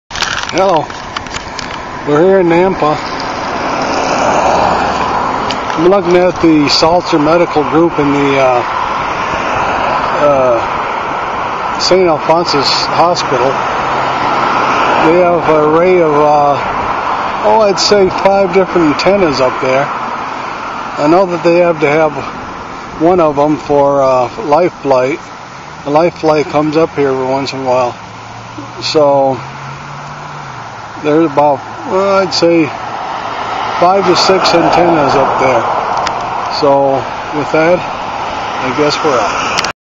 an array of antennas on the saint alphonsus hospital building. life flight comes here often
these antennas are on saint alphonsus hospial building. one of these antennas talk to life flight.